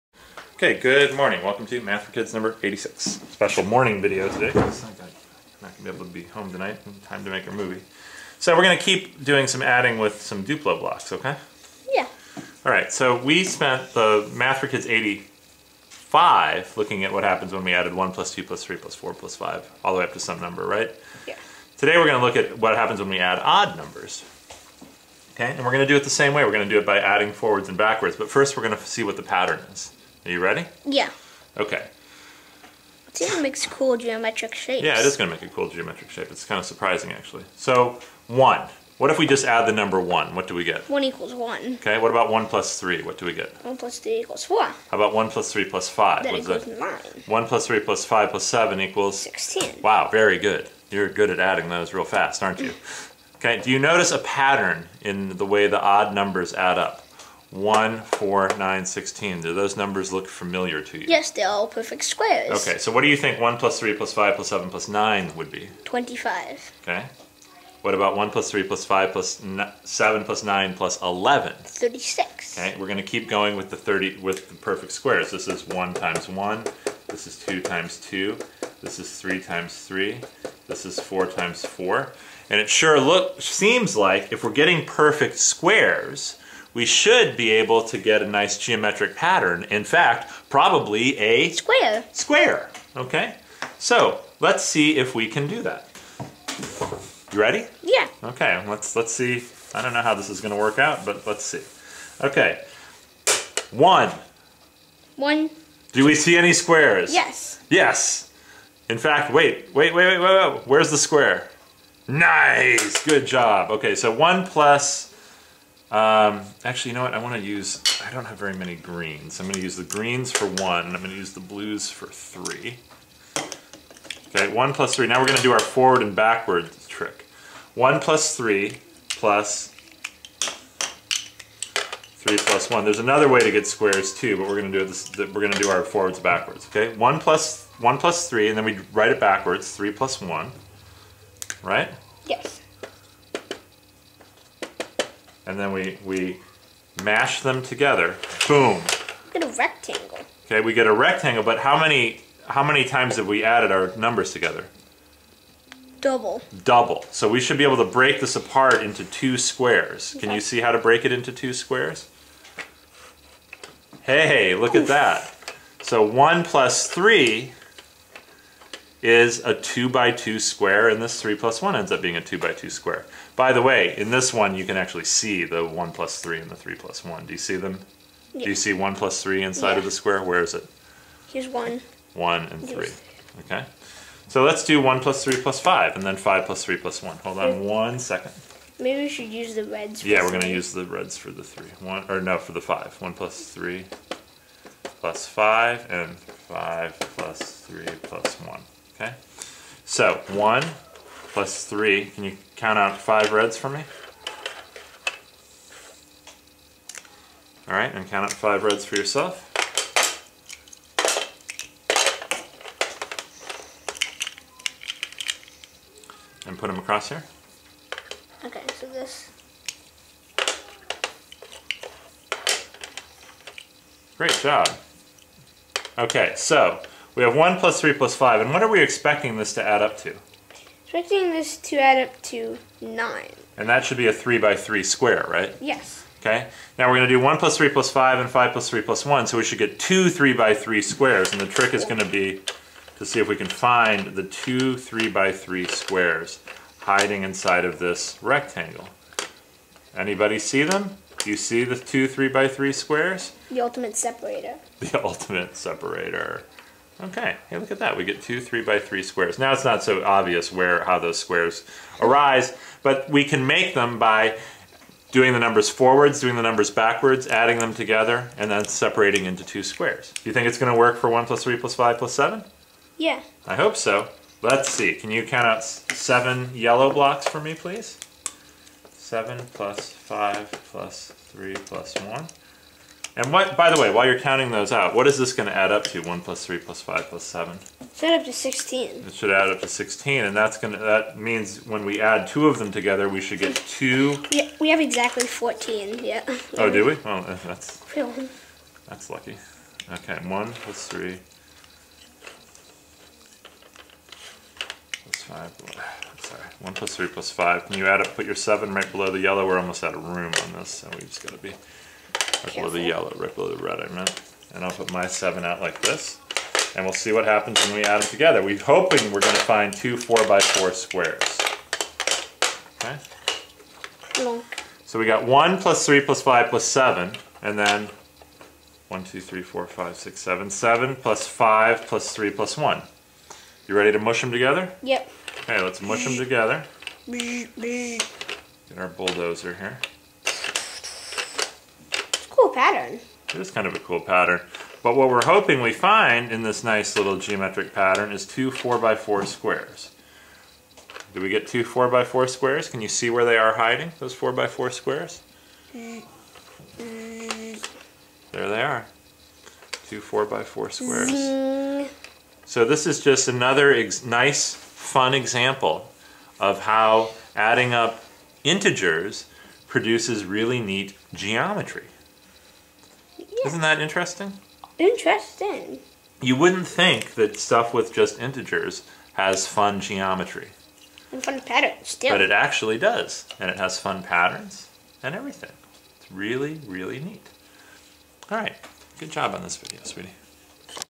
MathForKids86
Fun video looking at a surprising connection between arithmetic and geometry.  In this video we look at the sum of odd integers.